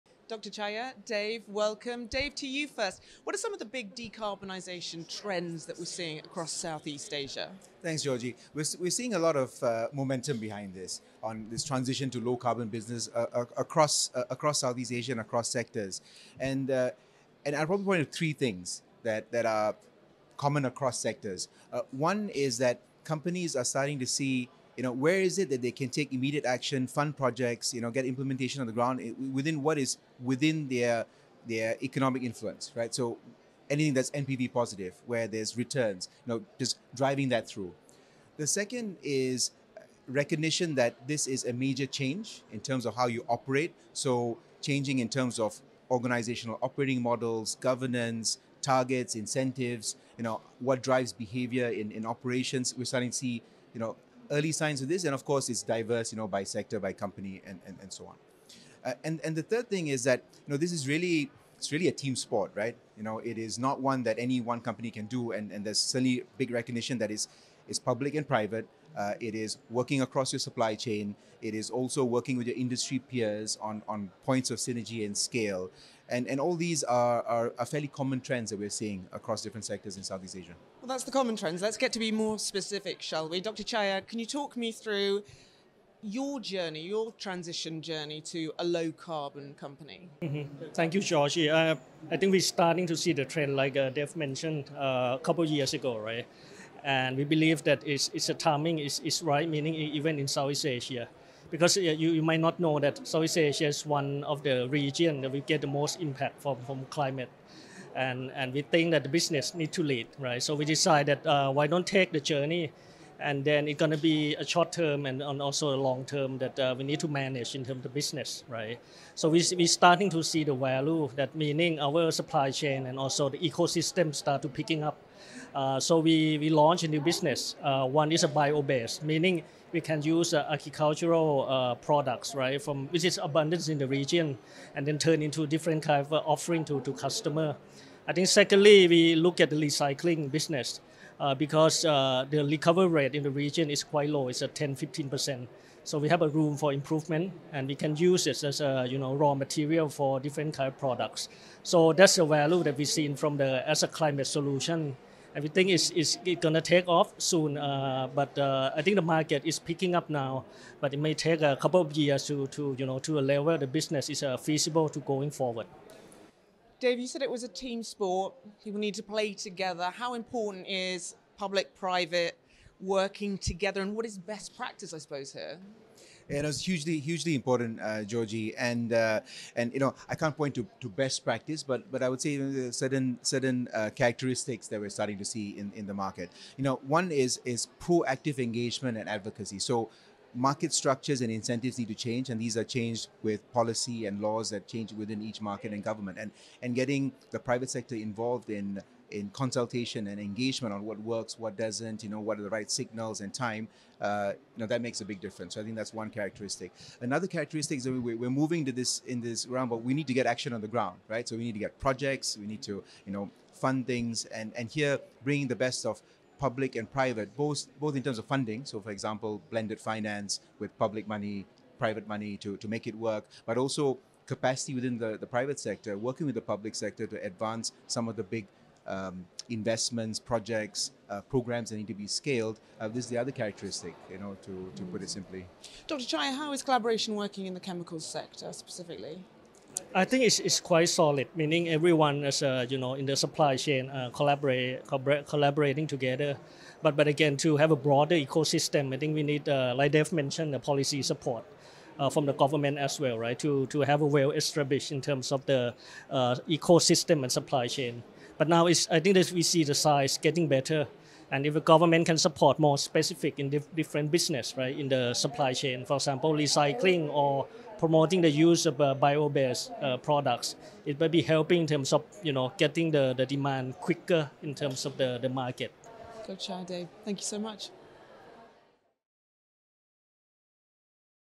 Decarbonization Trends and Collaborative Solutions
Discover how Southeast Asia businesses, including those in the chemicals sector, are navigating the shift to low-carbon models through collaboration, policy support, and innovation.